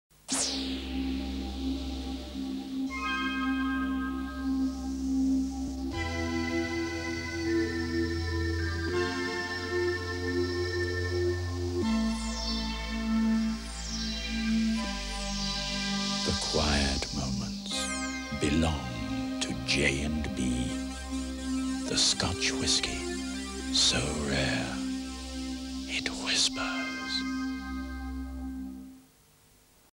J&B Scotch Whiskey - 1984 Australian TV Commercial
Hang on, you hear the whiskey whispering to you when you drink it? Might need to give the distilling equipment a clean.

Recorded during a broadcast of The Professionals from ATN-7 Sydney on Thursday 4 October 1984 at 9:30pm.

Thanks to Petarkco for the tape!